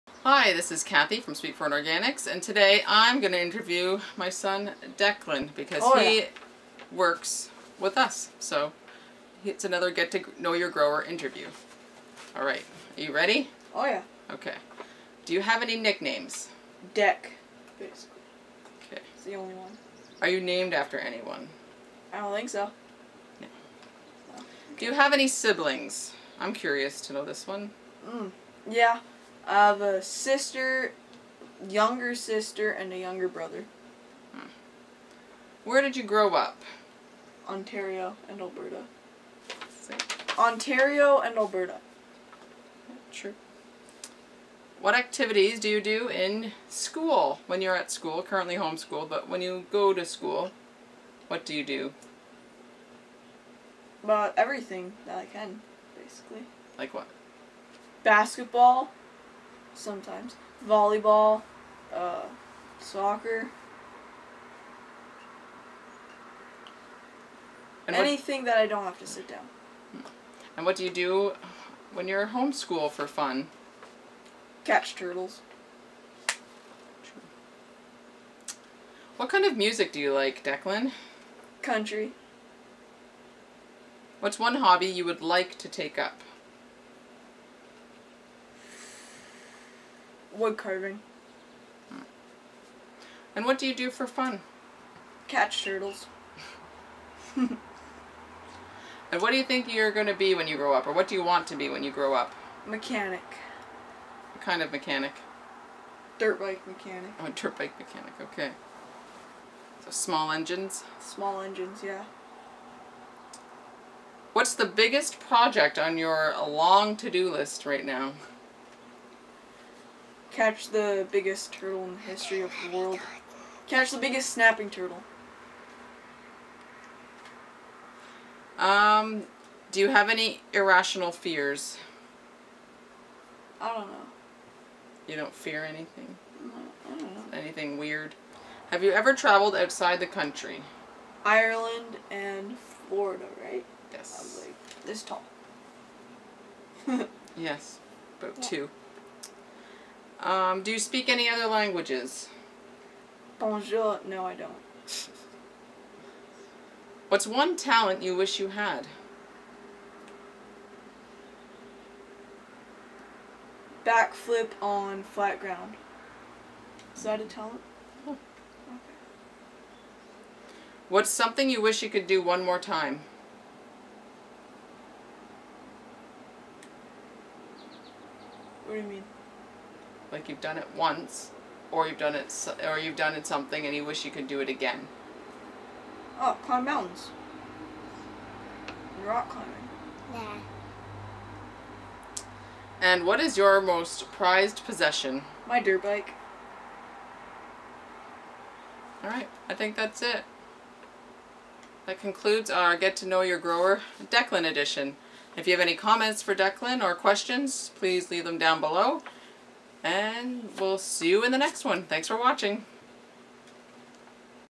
Get To Know Your Market Garden Grower | Declan
In this video, I interview our very hard-working teammate (and son), Declan.

Declan is 12 years old and is a big help around the farm as well as the farmer’s market.